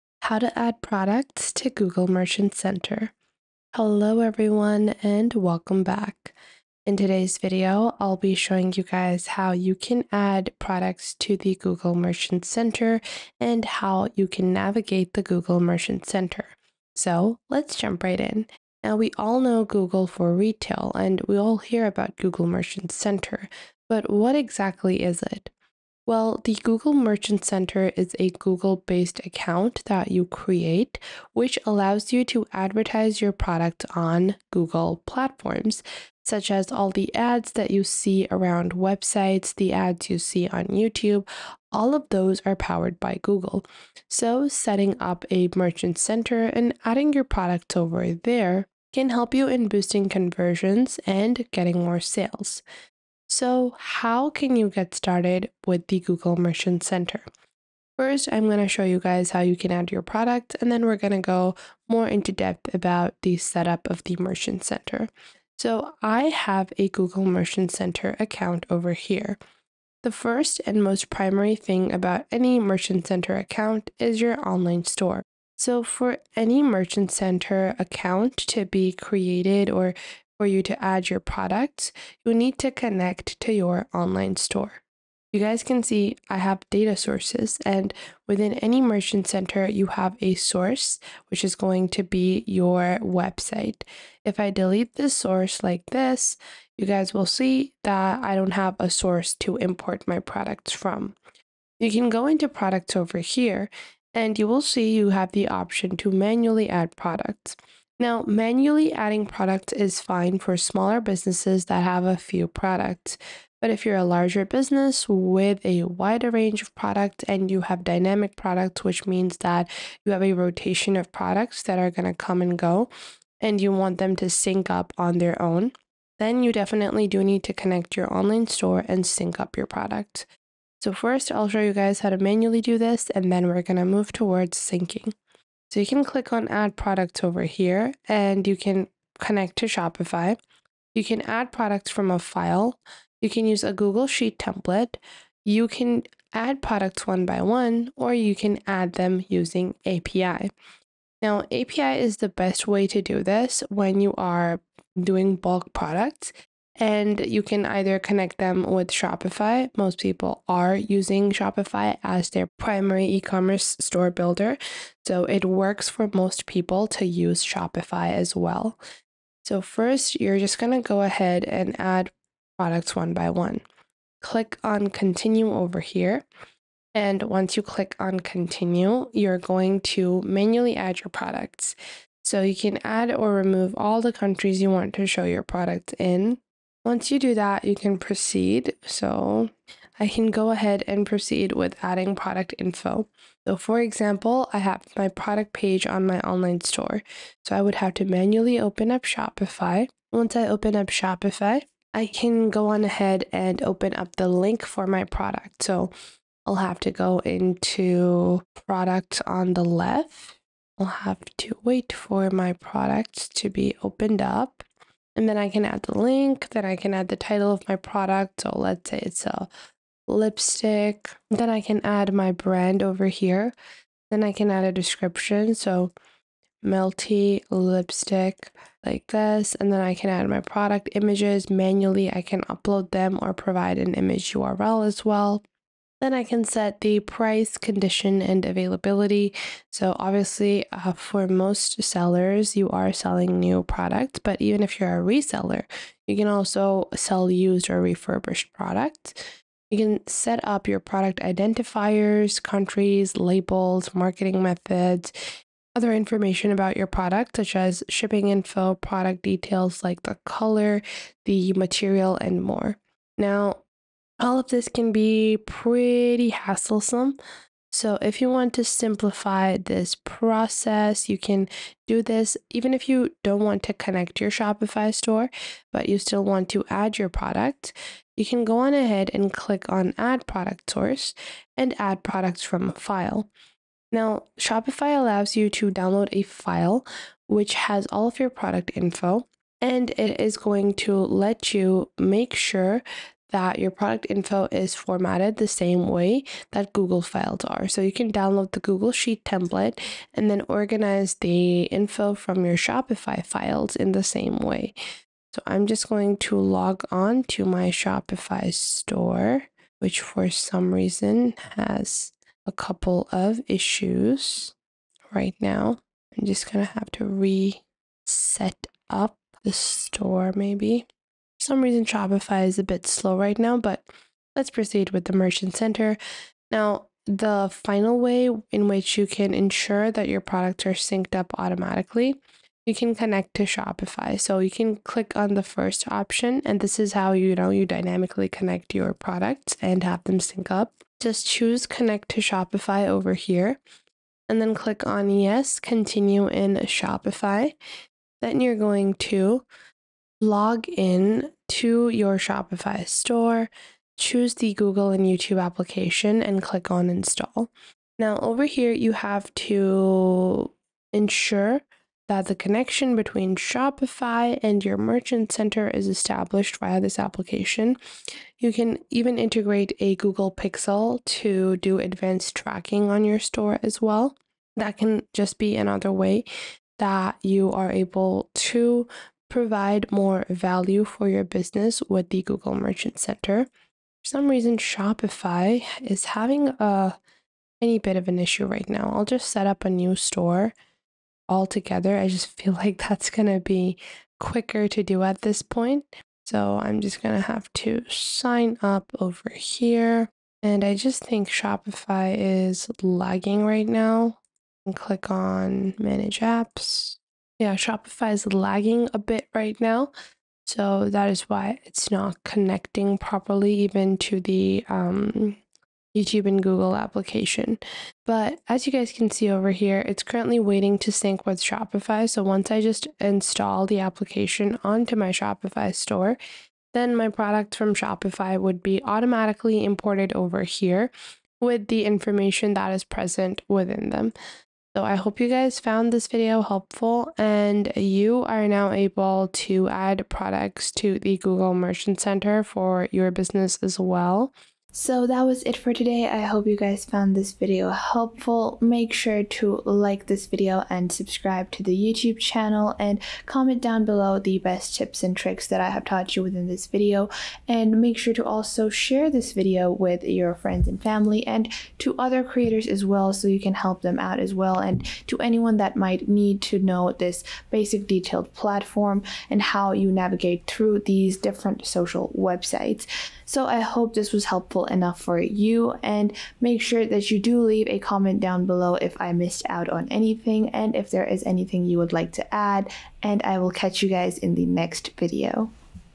How To Add Products To Google Merchant Center (2026)
To add products to Google Merchant Center, begin by logging into your account. Navigate to the "Products" section and select “Add Product.” You can choose to upload a single item or bulk upload using a feed. Ensure your product data complies with Google’s specifications, including details such as title, description, price, and availability.

Utilize structured data markup on your website to enhance visibility. After entering the necessary information, review and submit your products for approval. Regularly monitor your account for any issues or updates to maintain compliance and optimize your product listings for better performance in Google Shopping.